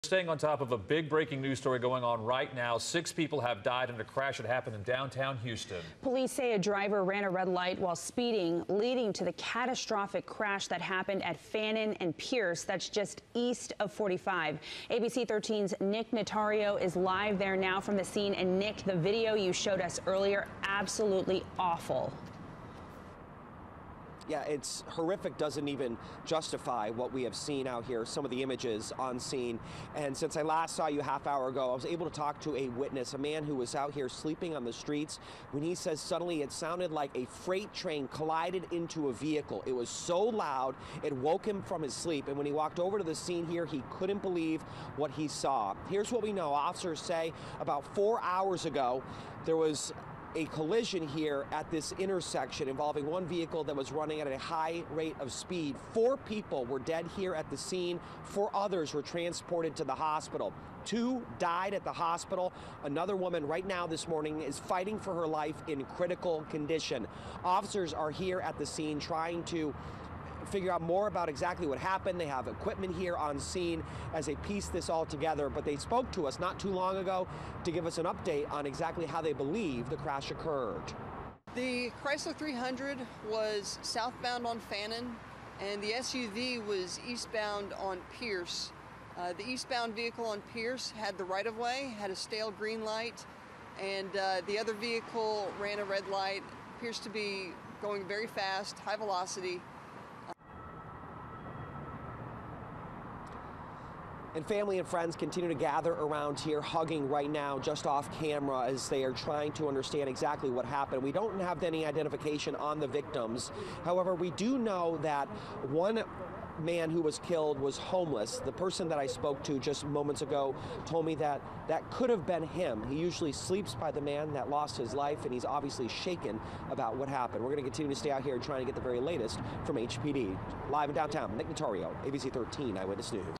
Former NFL star D.J. Hayden among 6 killed in Texas crash, police say
Six people died from a horrific overnight crash in Texas early Saturday. Former NFL and Oakland Raiders player D.J. Hayden is among the victims.